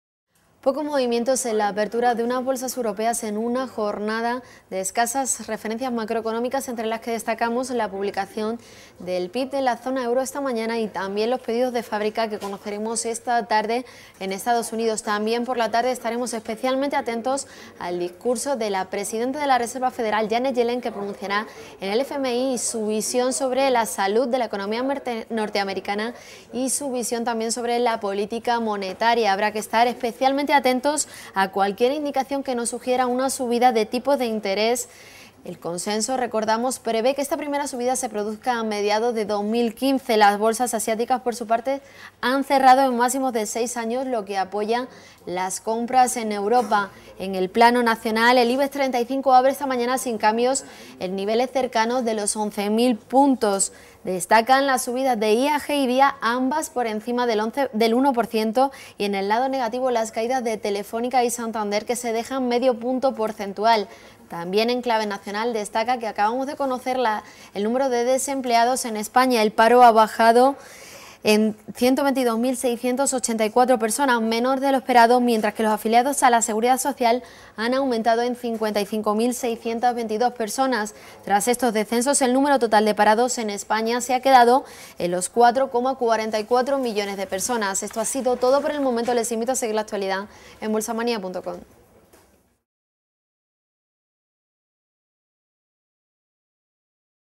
Yellen tiene la llave de nuevas subidas
Esta tarde la presidente de la Reserva Federal (Fed), Janet Yellen, pronunciará un discurso en el Fondo Monetario Internacional (FMI) y el mercado estará especialmente atento a cualquier referencia a una posible subida de tipos de interés.

Los inversores realizarán un análisis exhaustivo de su visión sobre la "salud" de la economía de Estados Unidos; sobre todo, a la espera de la publicación del siempre influyente Informe de Empleo que conoceremos mañana.